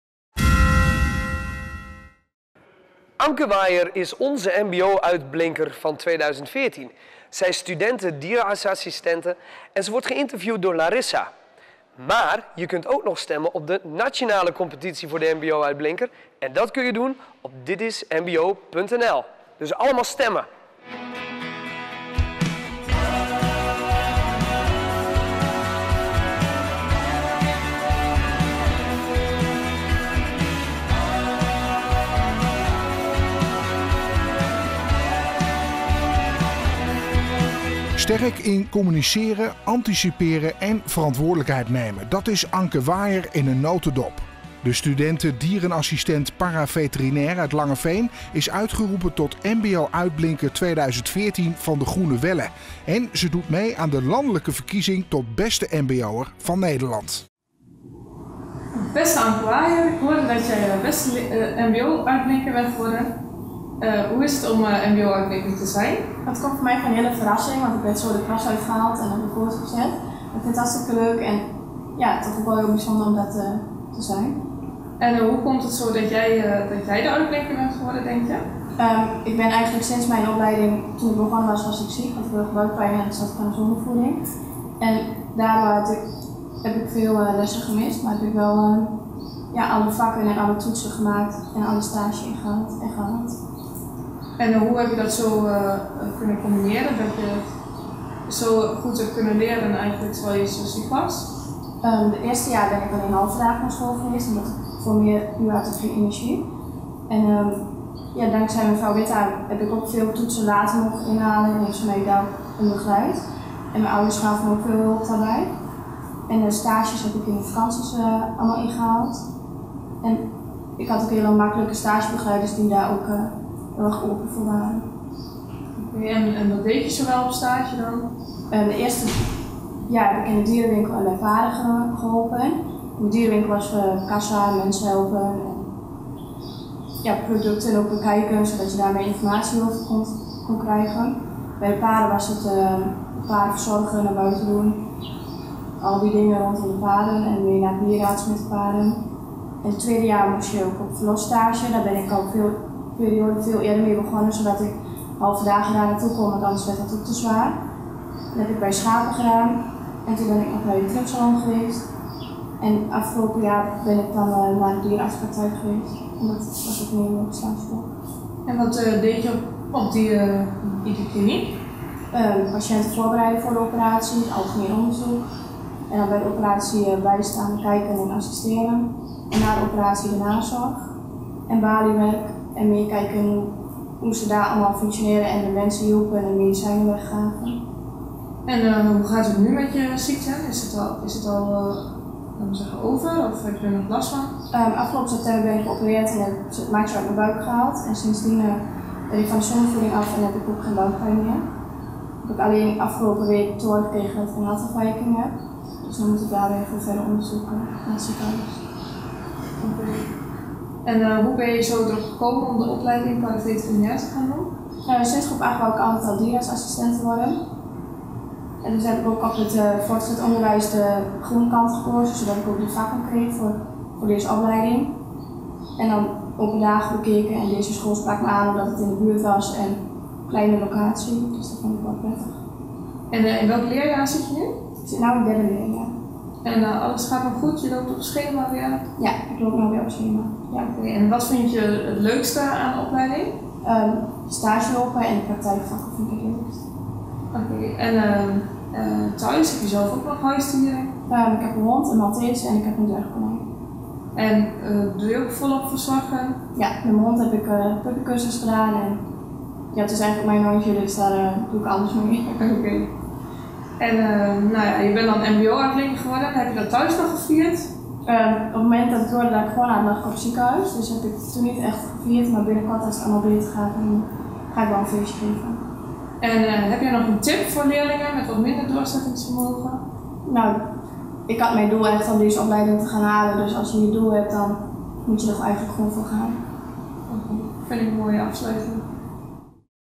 GREEN MAGAZINE 5 | De Groene Welle MBO | MBO Uitblinker 2014
MBO UITBLINKER! Gesprek met onze MBO Uitblinker van 2014.
Anke Waaijer is dit jaar de uitblinker en jullie kunnen nog op haar stemmen. Ga naar: ditismbo.nl.